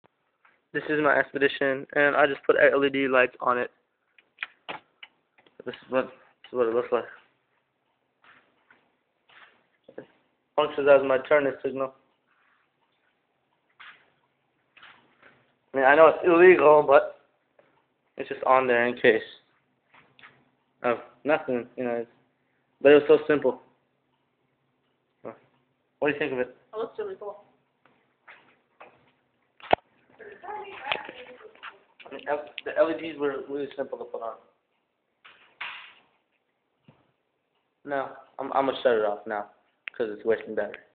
ford expedition led strip installation
this is my ford expedition 1998 with blue leds as turn signals

i have looked everywhere online to learn how to install these led strips, but i couldnt find any.
so i am going to make a tutorial on how to install these type of leds

this is just a preview of what i will show you guys how to install in a couple later videos